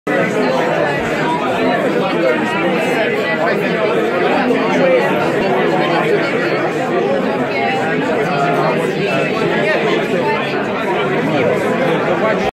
Schwarz Etienne Manufacture - La Chaux-de-Fond Flying Petite Seconde Retrograde Tourbillon
Schwarz Etienne Manufacture - La Chaux-de-Fond Flying Petite Seconde Retrograde Tourbillon at Wine&Time Pop Up Basel 2017